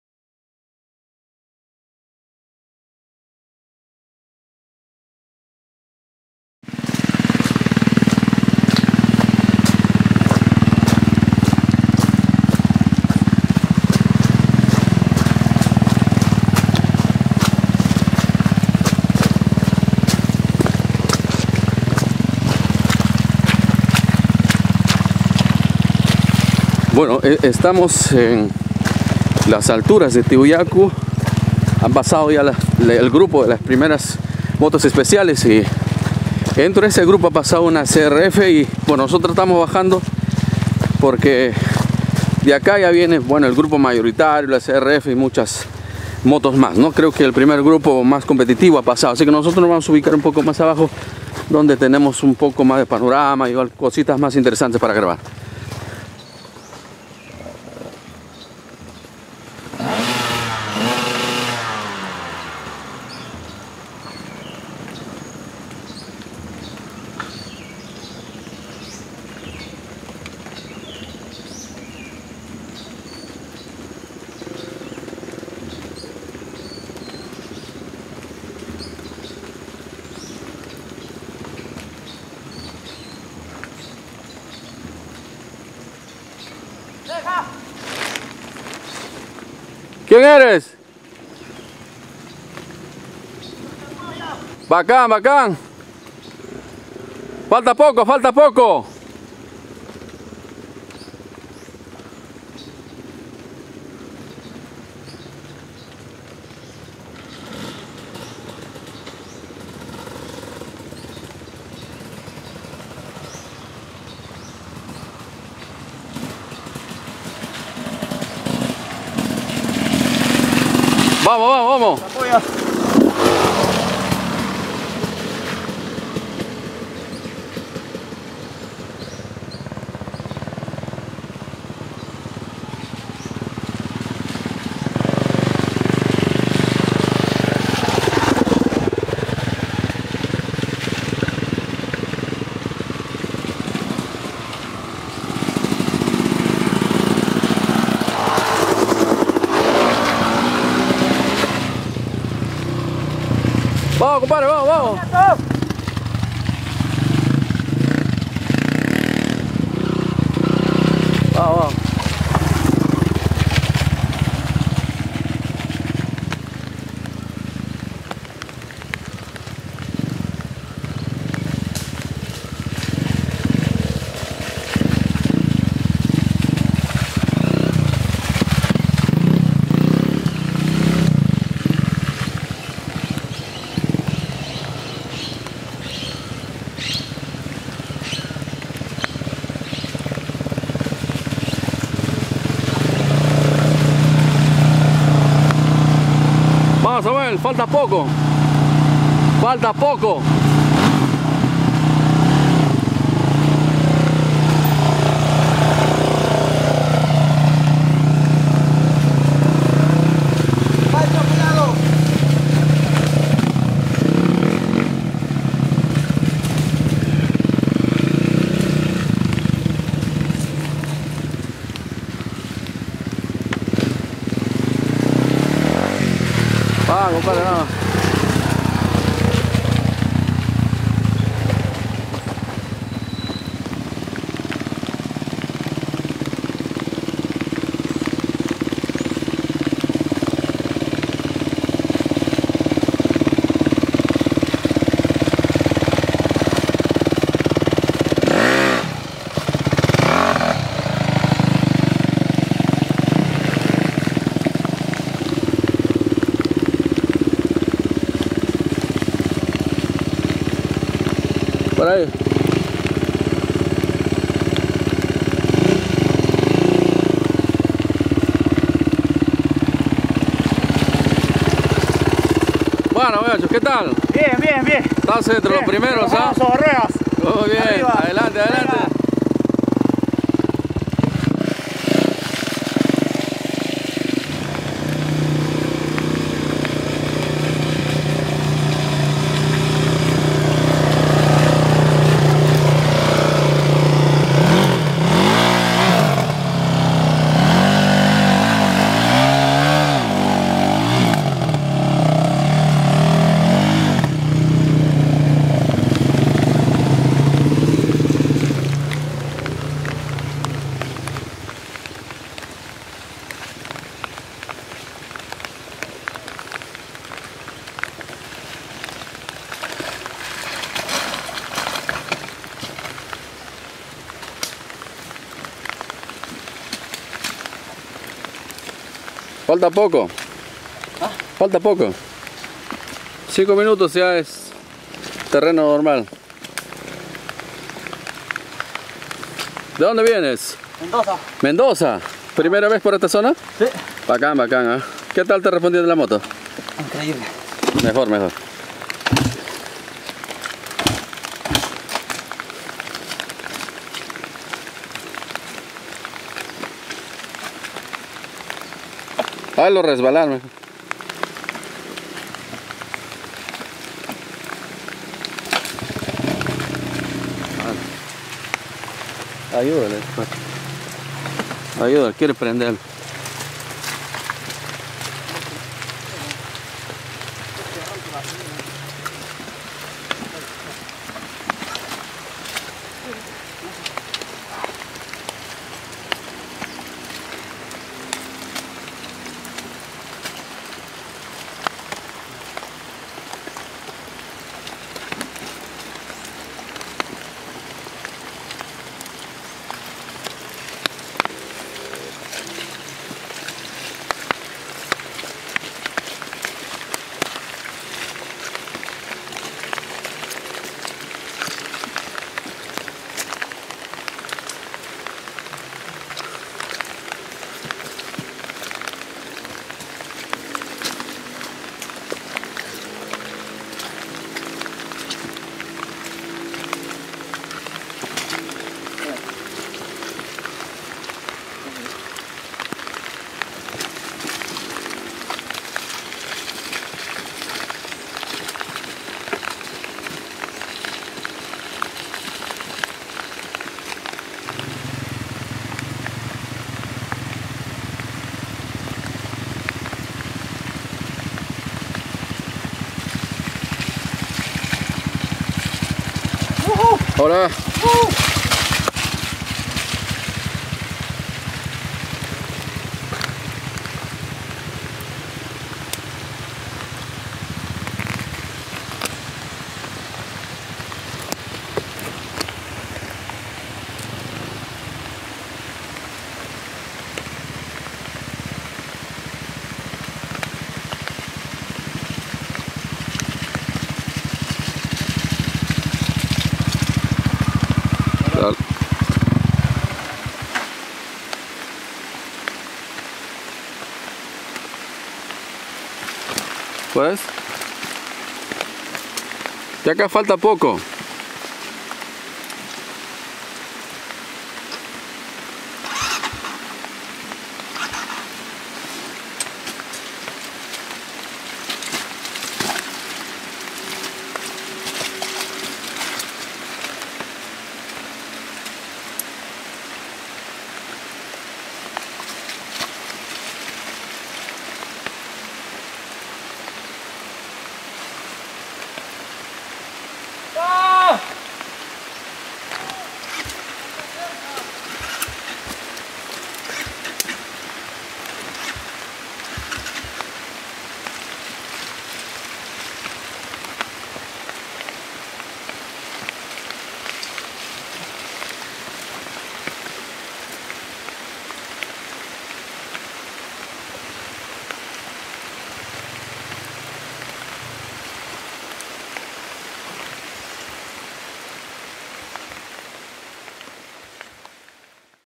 SACHA DAKAR 2016 TIOYACU SEGUNDA PARTE
ESTE ES EL SEGUNDO VIDEO DE LA ZONA DE TIOYACU, QUE DAN DOS MAS , LUEGO PASAMOS A LOS OTROS SECTORES